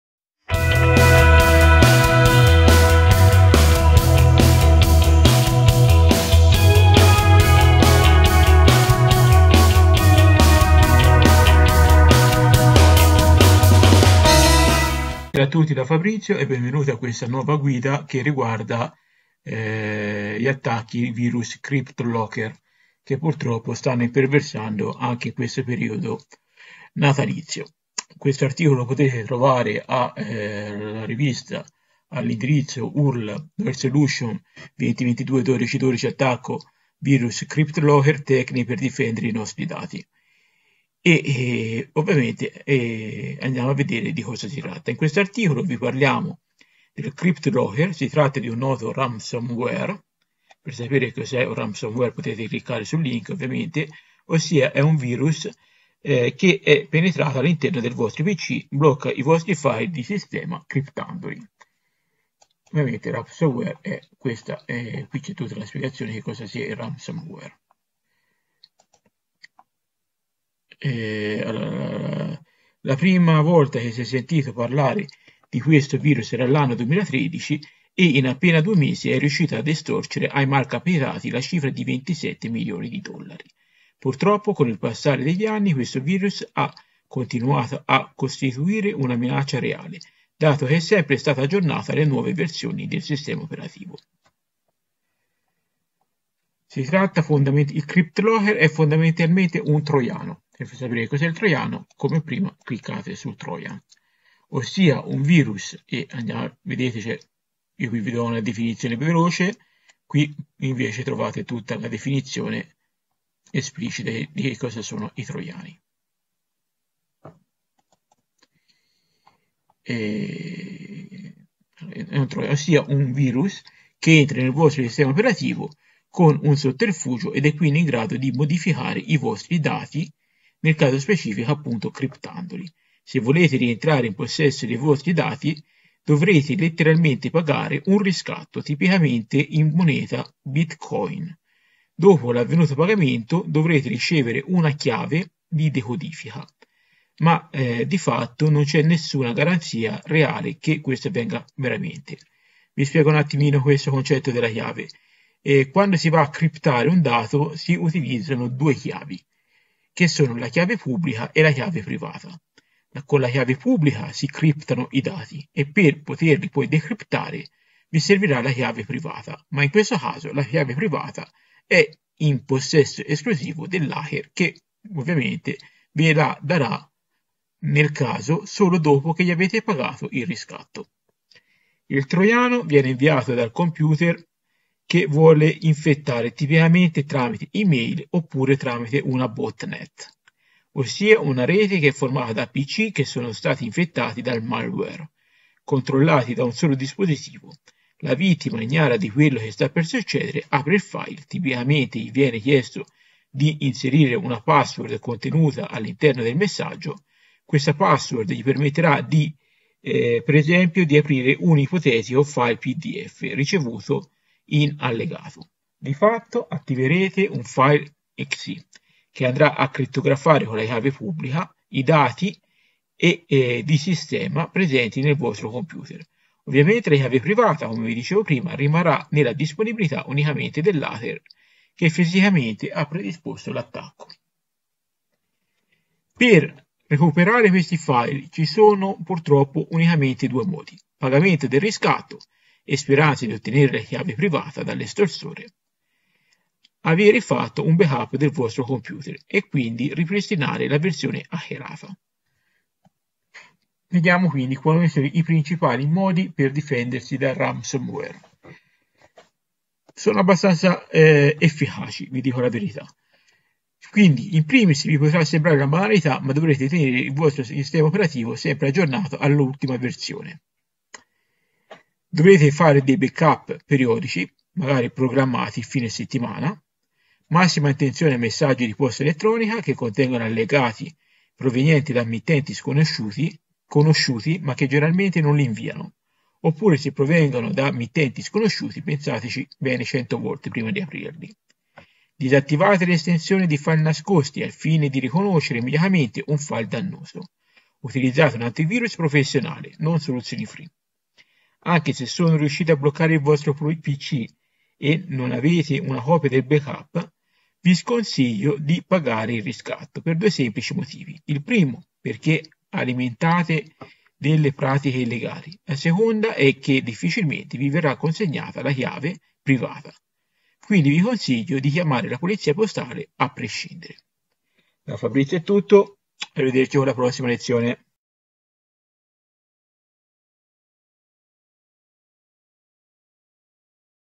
Come difendersi dal Cryptolocker Ransomwere
In questo articolo vi spieghiamo come difendersi dal Ransomware Criptolocker. Si tratta di un virus trojan che si insinua all'interno del vostro PC tramite email in genere. Una volta penetrato  cripta tutti i vostri dati tramite una chiave pubblica, e per avere di nuovo i vostri dati dovrete pagare un riscatto per ottenere la chiave privata.